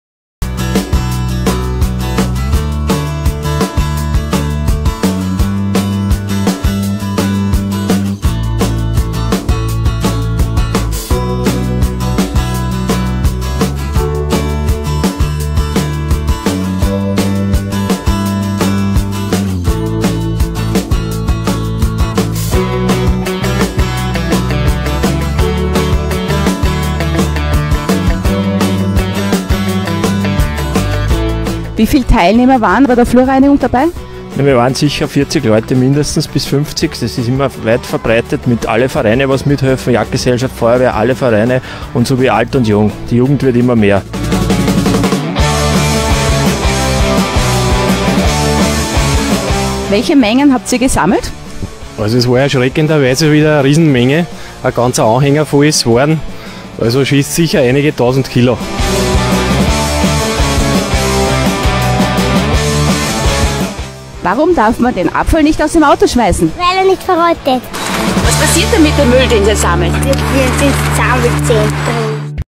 Stopp Littering wir halten Niederösterreich sauber
Die Flurreinigungen im Frühjahr sind landesweit die größten Umweltaktionen in Österreich. Tausende Menschen räumen die Abfälle des Winters von anderen weg. Ein dickes Dankeschön an alle Freiwilligen